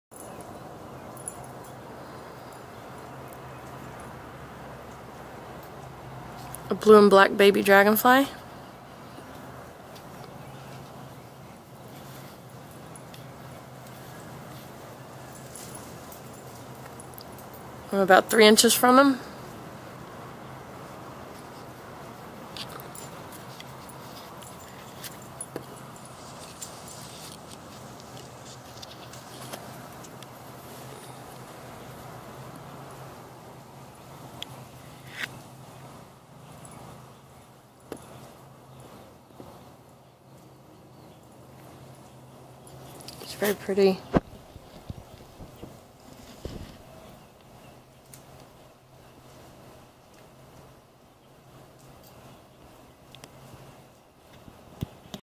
VERY PRETTY BABY DAMSELFLY
This is a beautiful black and blue young dragonfly! i was 3 inches from him with the camera! this is as close as you will get! I hope you enjoy seeing this calm pretty damselfly up close. I could not believe he stayed so still.